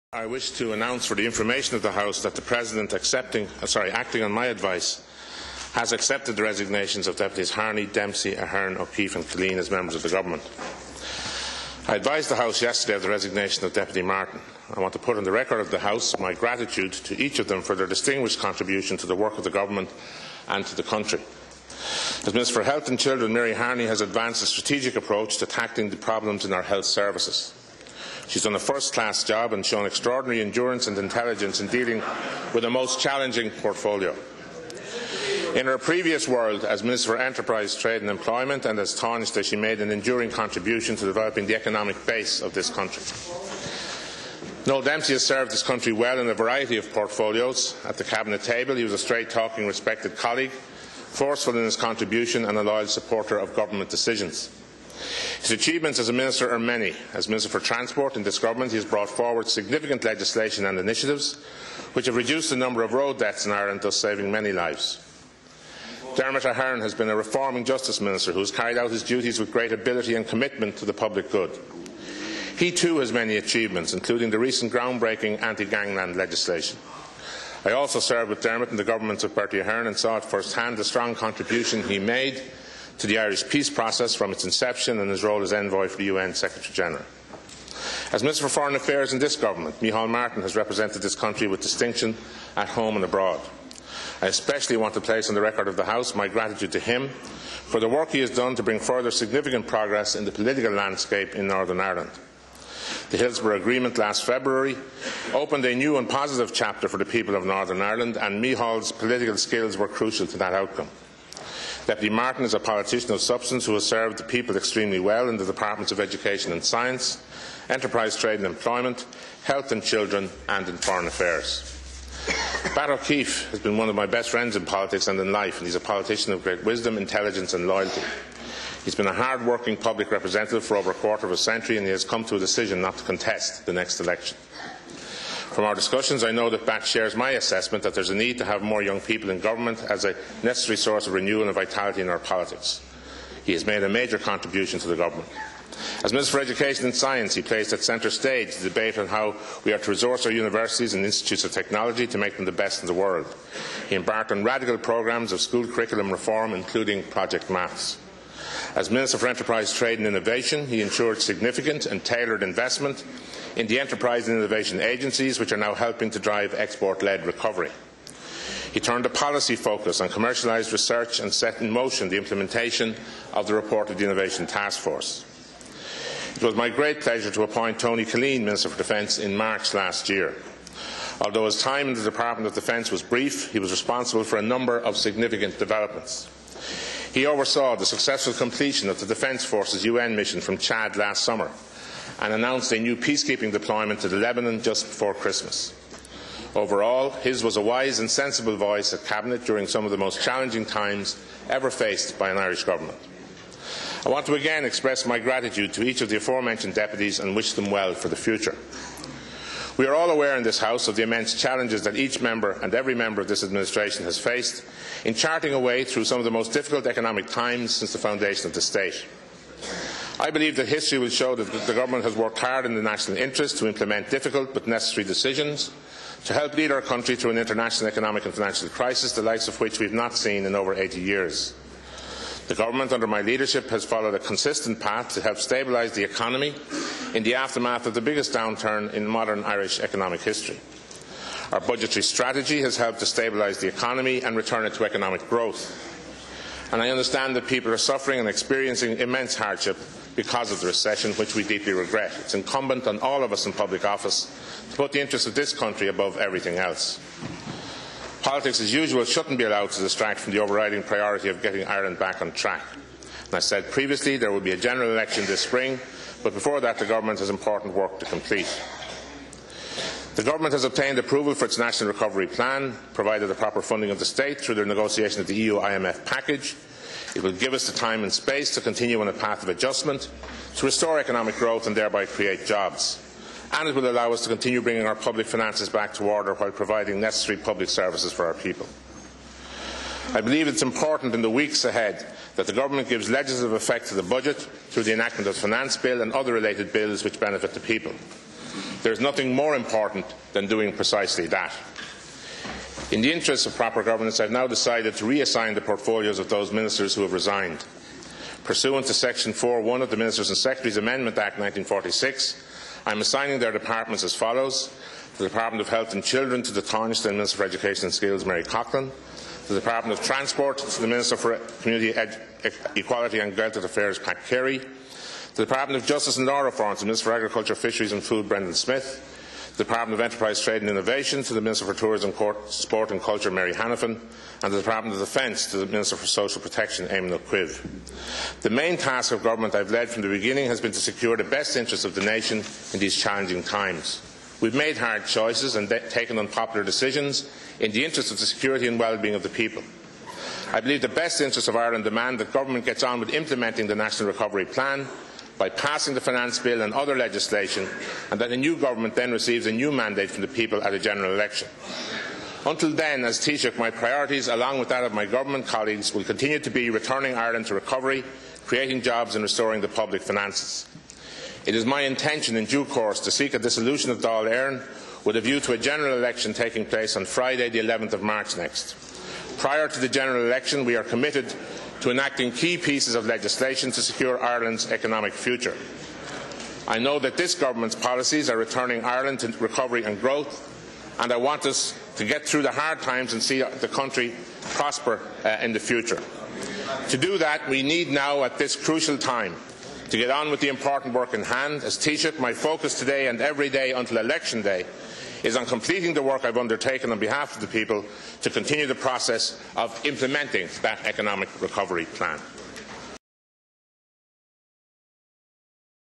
Brian Cowen Calls for Genereal Election on March 11th 2011
for coverage of all political developments as the Taoiseach says the General Election will take place on 11 March.